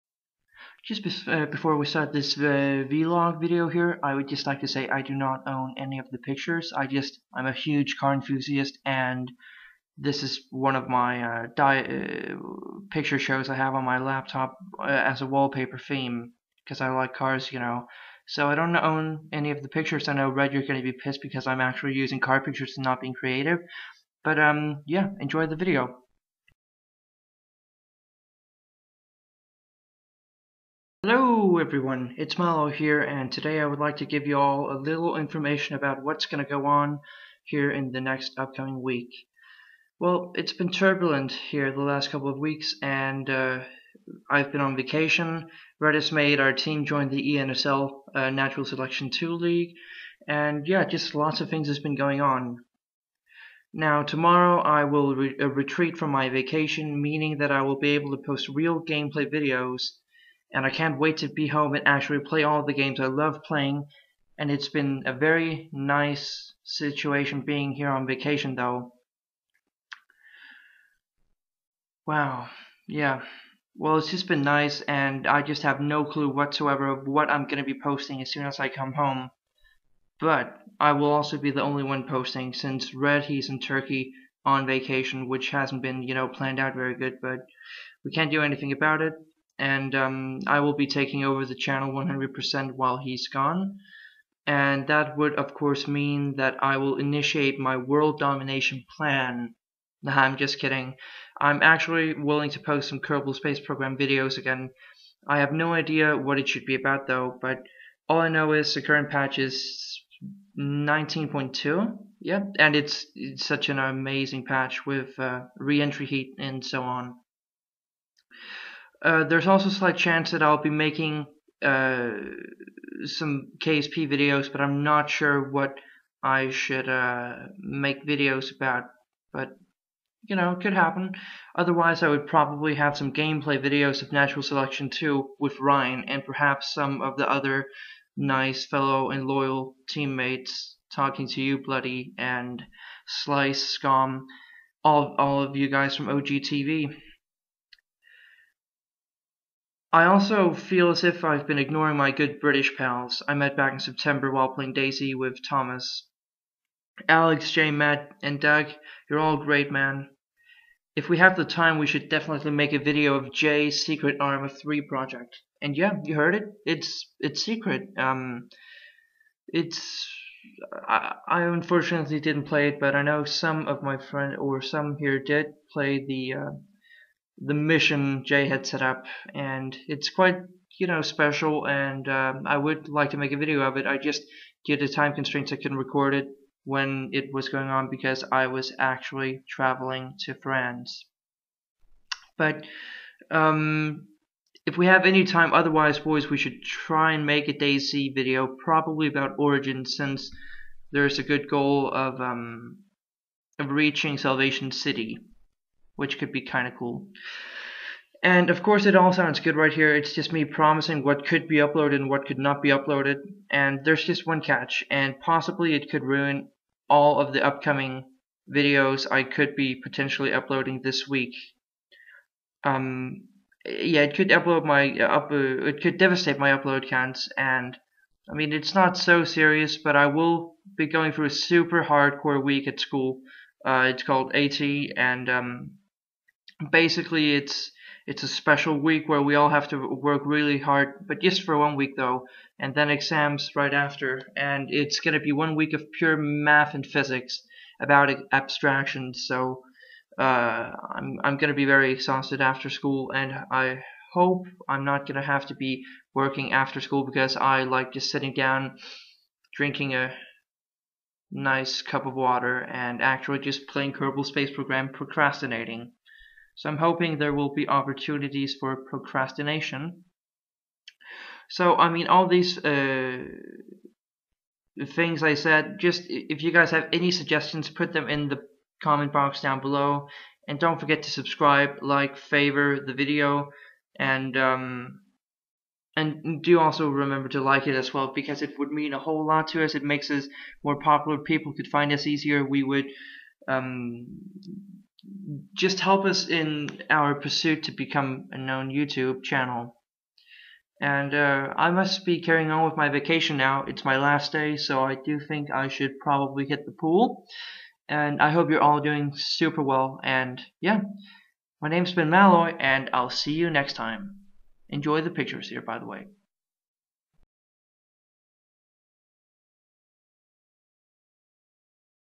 Just another Vlog
Malloy just talks without rambling as much this time. Now that the vacation is ending, it is soon time to upload real video´s again. Can he handle it?